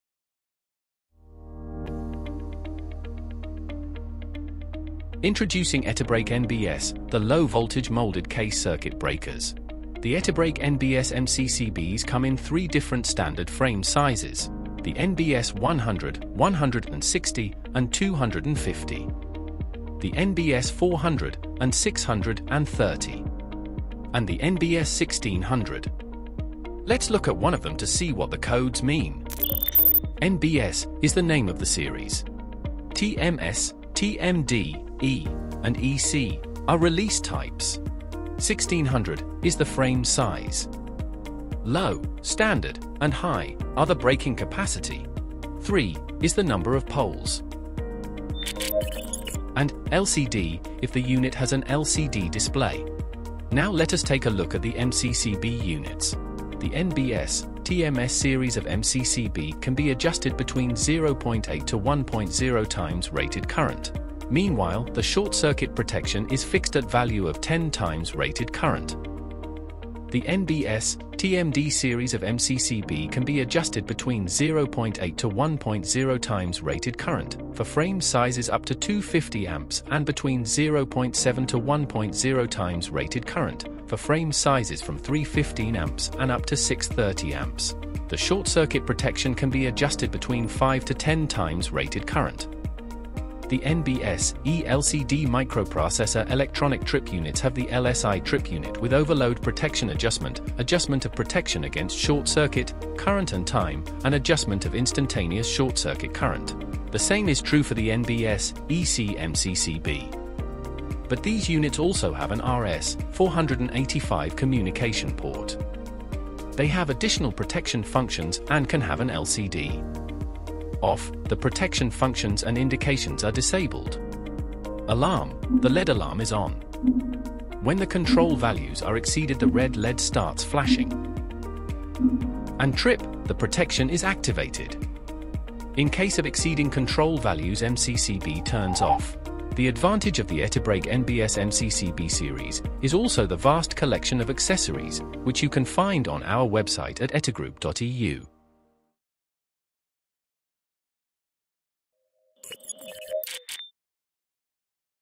Introducing ETIBREAK NBS – A Versatile MCCB Solution
ETI is introducing a new series of low voltage MCCBs, the ETIBREAK NBS, with a wide range of framesizes, protection types (thermal-magnetic or electronic) and accessories.